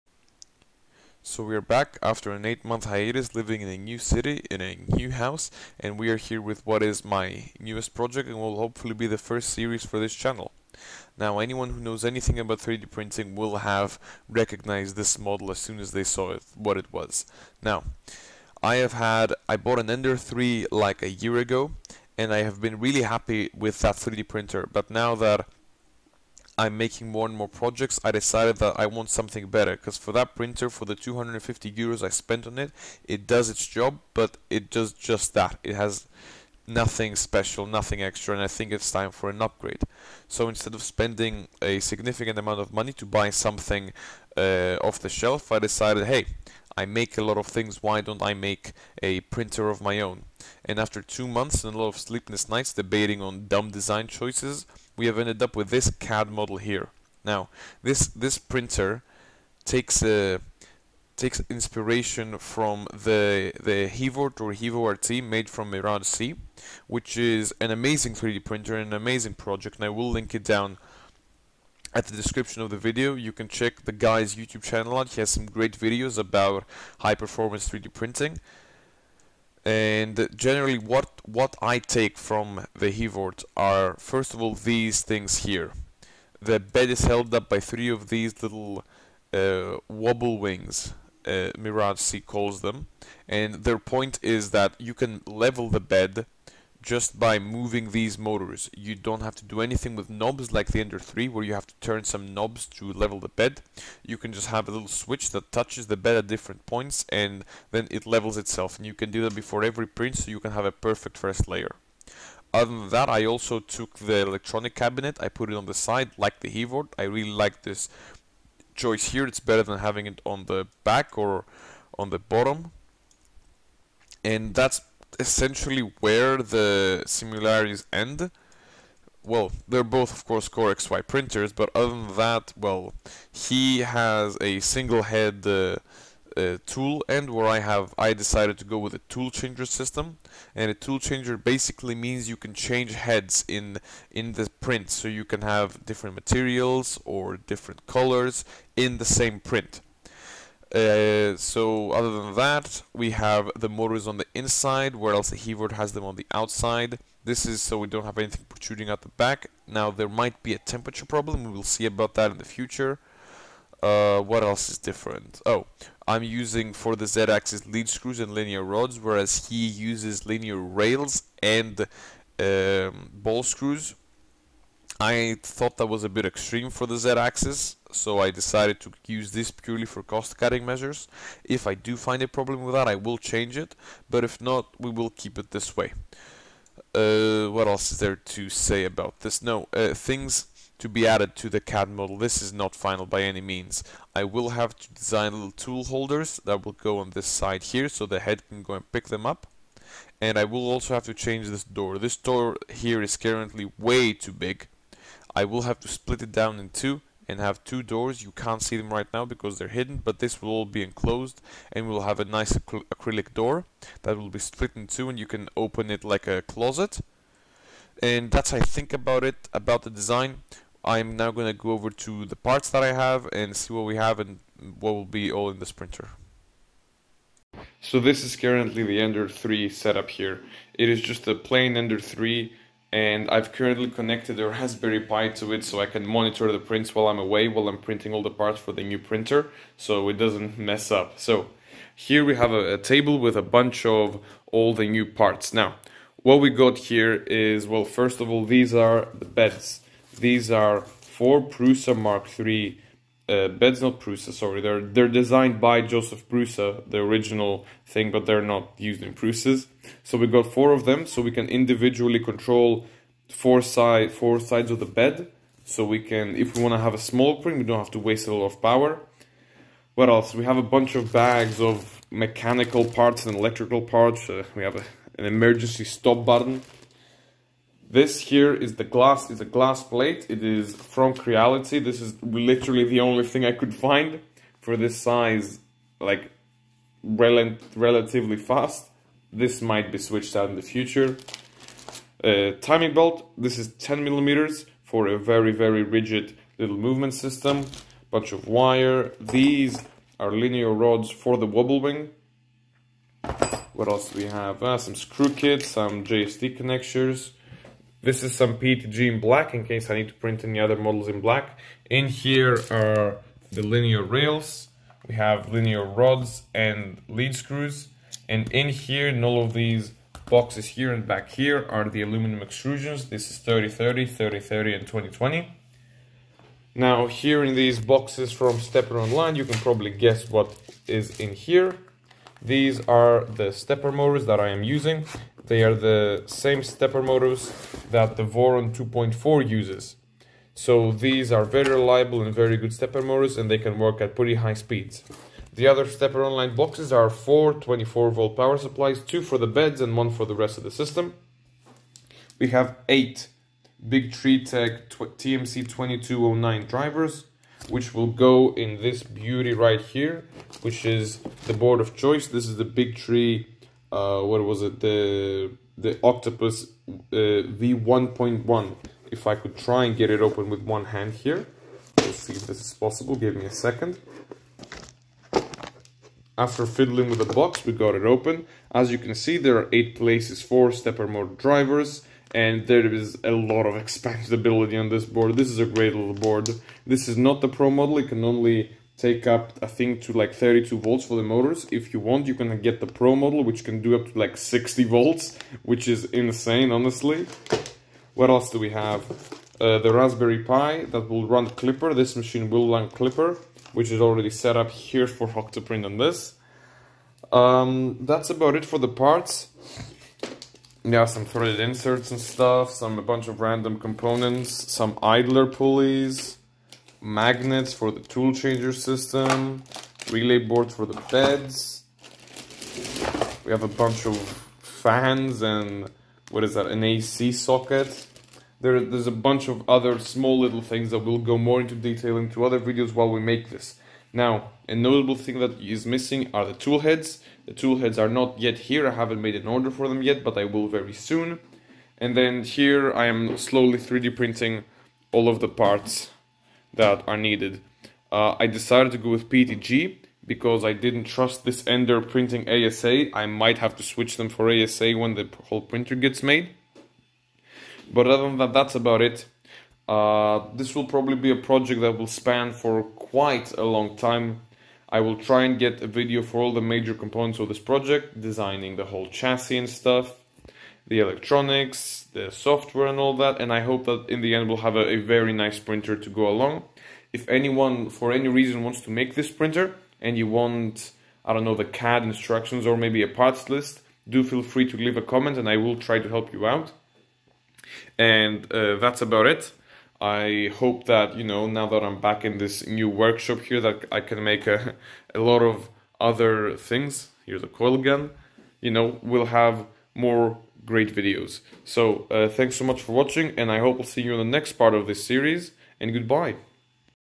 DIY big format 3D printer with toolchanger--Part 1
This is the first part of my new series where i design and make my own 3D printer featuring a 400*400*500 print area, 3030 extrusions for the chasis,a fully DIY toolchanger system,linear rails for x and y axis,area controled heated bed(4*4),full enclosure and many more exciting features.In this first part we take a look at the cad model and the parts that will be used.Next episode we will build the chasis.